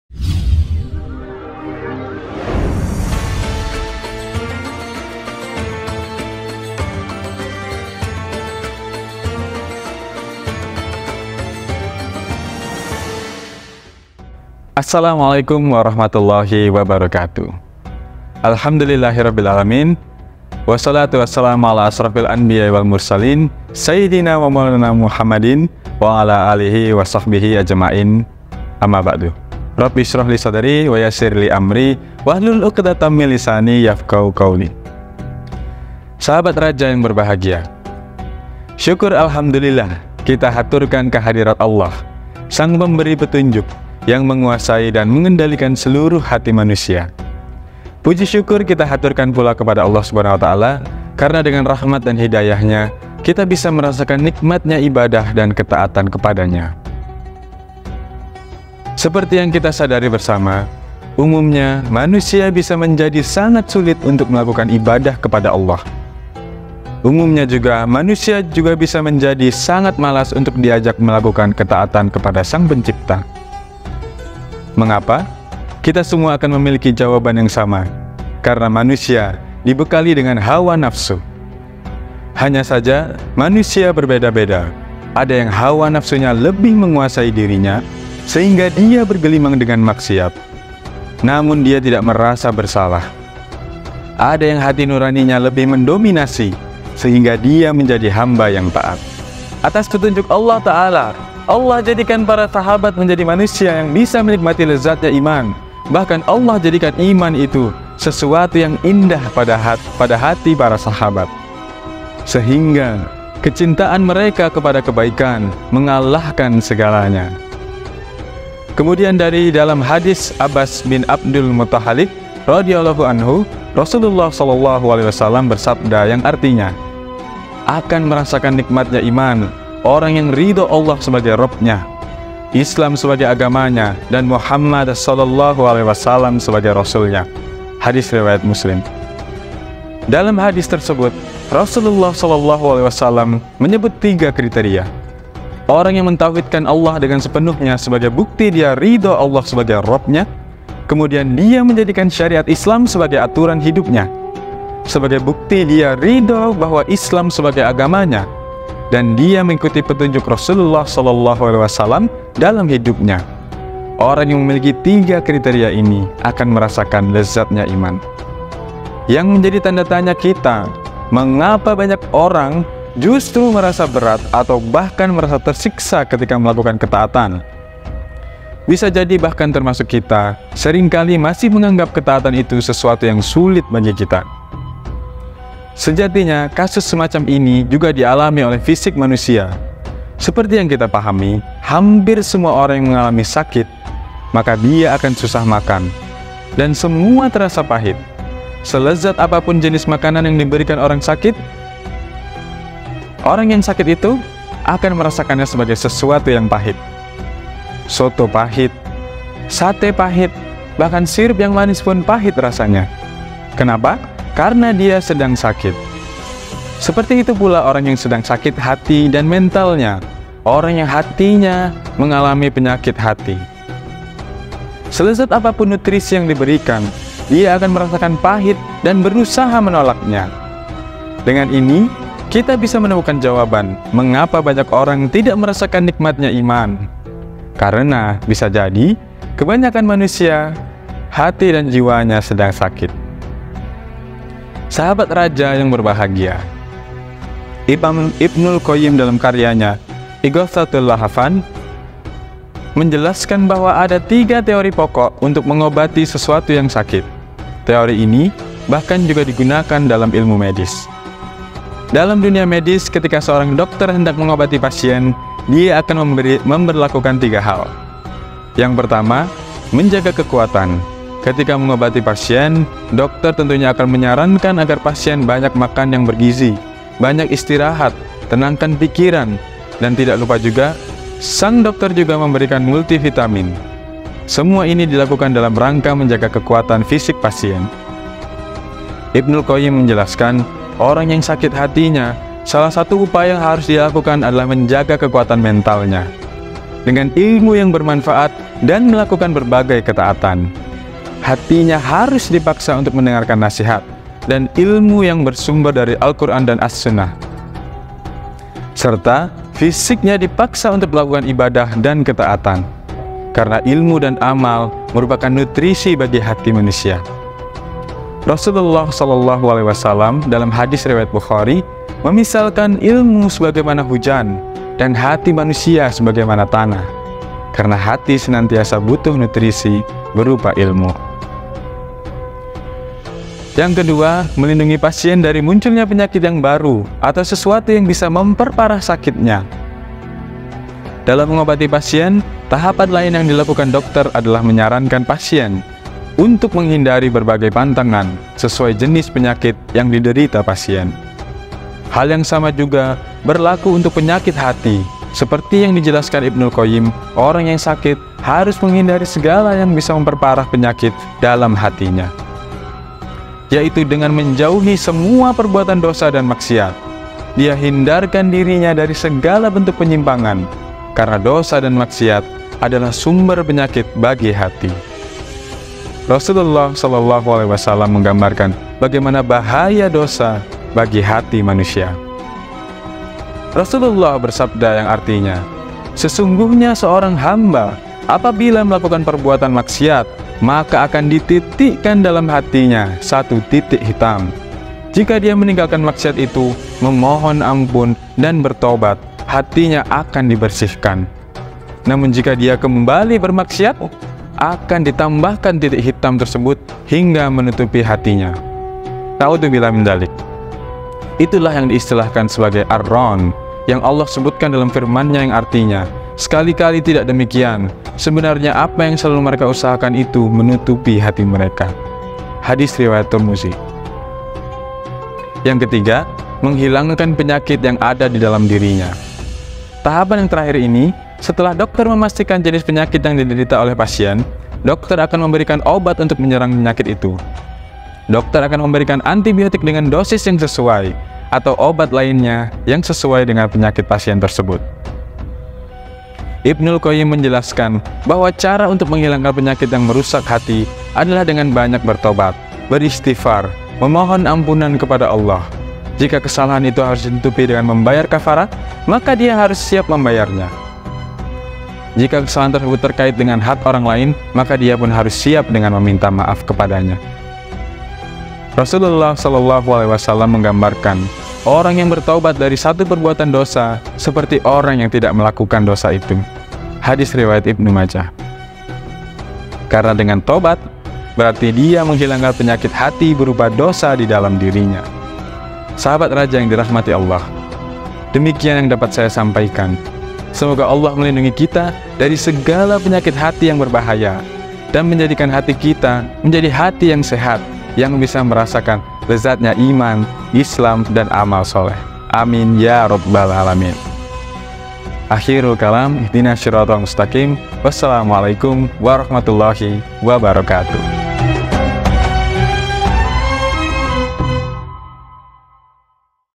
Kultum Ramadan 1445H Eps 28 - Ustadz Pandu Prasetyo A.P, S.Pd.
Kultum Ramadan 1445H Perguruan Islam Raudlatul Jannah episode 28 bersama Ustadz Pandu Prasetyo A.P, S.Pd. dengan tema "Obat Penyakit Hati".